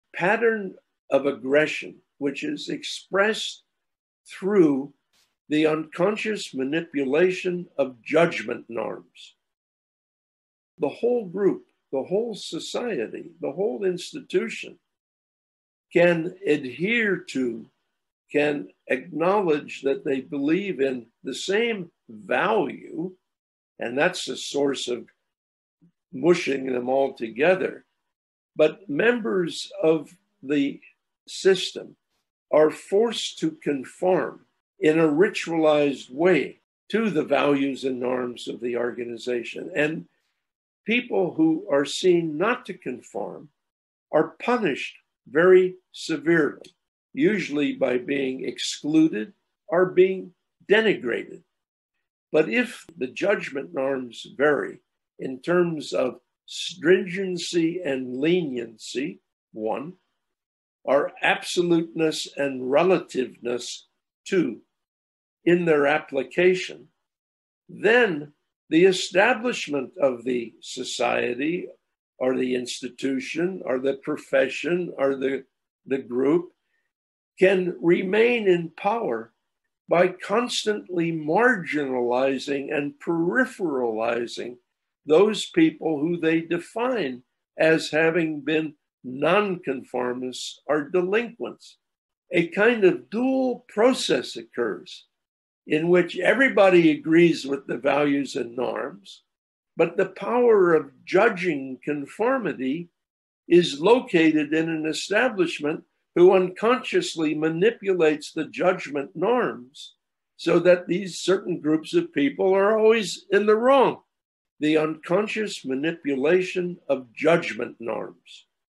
Earl Hopper The Unconscious manipulation of Judgment Norms
transcript:
pattern of aggression
which is expressed through the unconscious manipulation of judgment norms
the whole group the whole society the whole institution can adhere to, can acknowledge that they believe in the same value
and that's the source of mushing them all together
but members of the system are forced to conform in a ritualized way to the values and Norms of the organization
and people who are seen not to conform are punished very severely,
usually by being excluded or being denigrated,
but if the Judgment Norms vary,
in terms of stringency and leniency, 1
or absoluteness and relativeness, 2
in their application

then the establishment of the society or the institution or the profession or the group can remain in power,
by constantly marginalizing and peripheralized those people who they define as having been nonconformist or are delinquents

a kind of dual process occurs in which everybody agrees with the values and norms,
but the power of judging Conformity is located in an establishment who unconsciously manipulates the Judgment Norms
so that these certain groups of people are always in the wrong
the unconscious manipulation of judgment norms